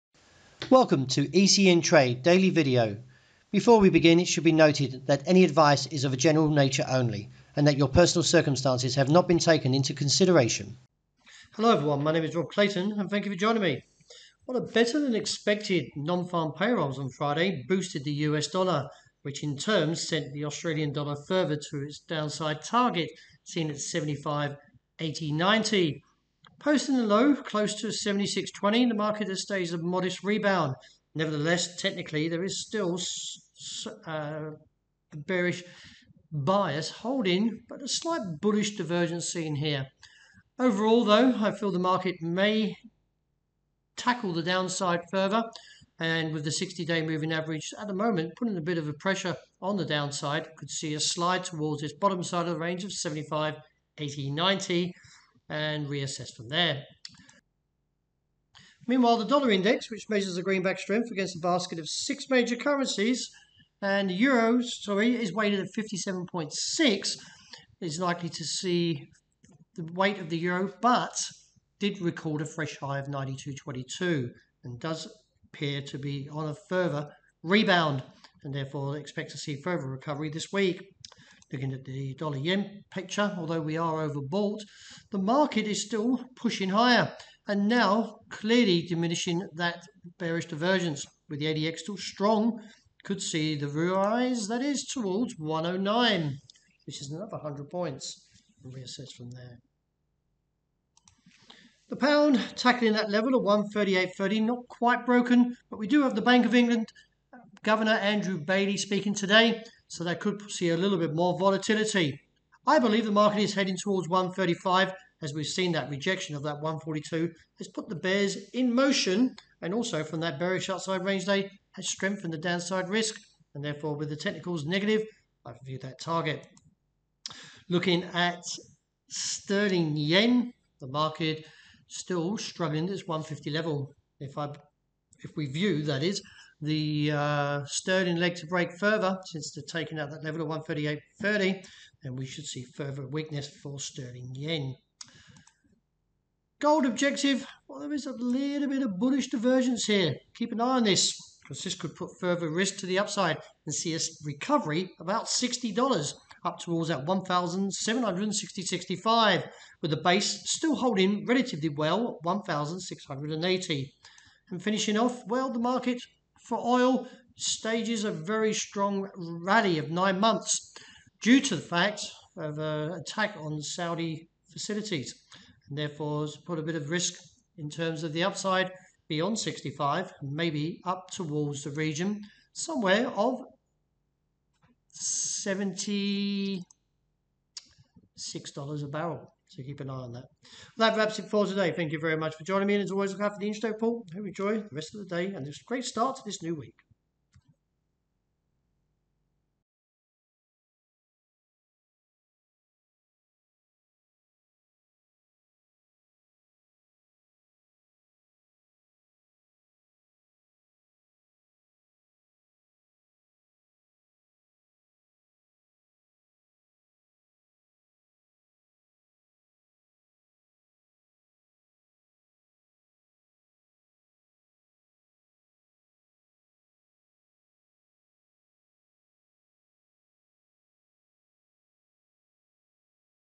ECNTRADE Webinar 2021 MAr 08
The Australian Dollar bearish sentiment increased on Friday and finished the week with modest losses after plummeting to an intraday low of 0.7621 and nearly claimed the region of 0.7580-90. Although we have seen a modest rebound since the low, it is assessed, we could see further challenges upon the recent lows.